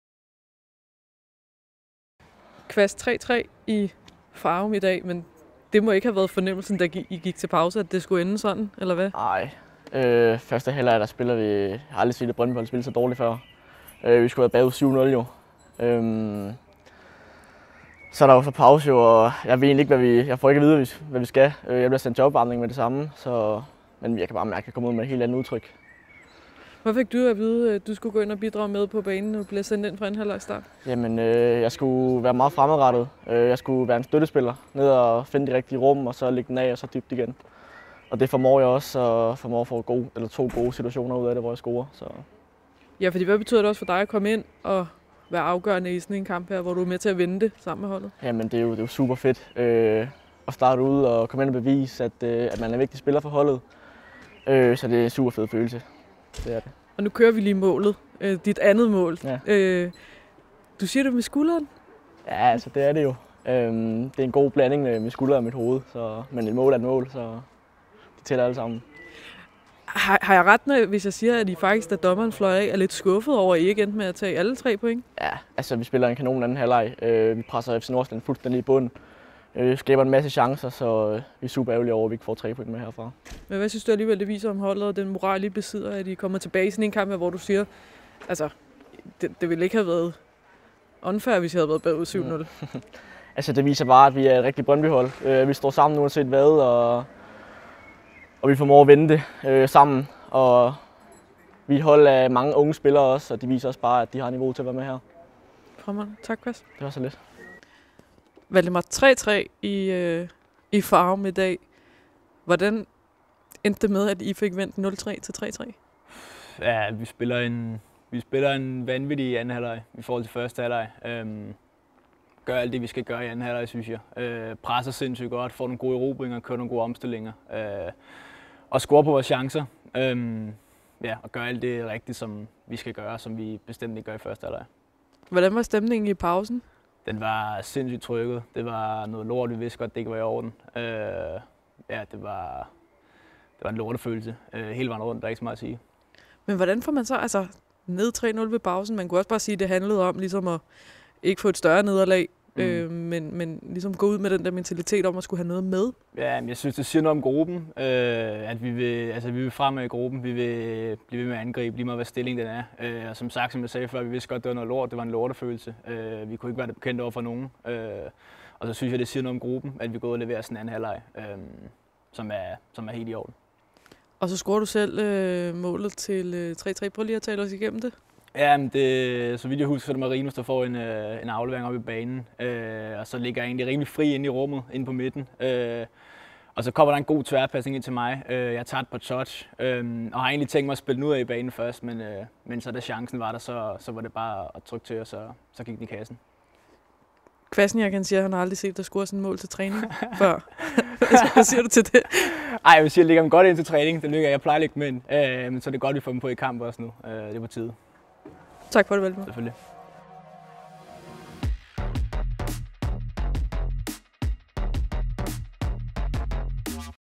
MÅL OG REAKTIONER FRA KWASNIAK OG KATTRUP: VI VISER GOD MORAL
Se mål fra Valdemar Kwasniak og Waldemar Kattrup, ligesom du her får reaktioner fra de to U/19-spillere efter 3-3-kampen mod FC Nordsjælland.